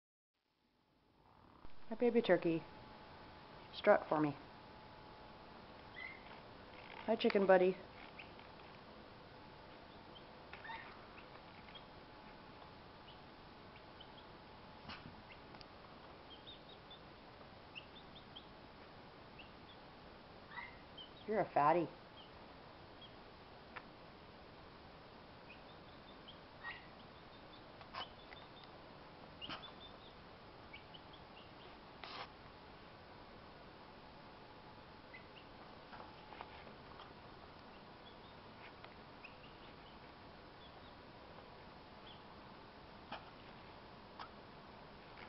Baby turkey strut
Young mixed breed turkey jake, and his hatchmate, a mixed breed bantam cockerel. Both are about 9 weeks old in this video.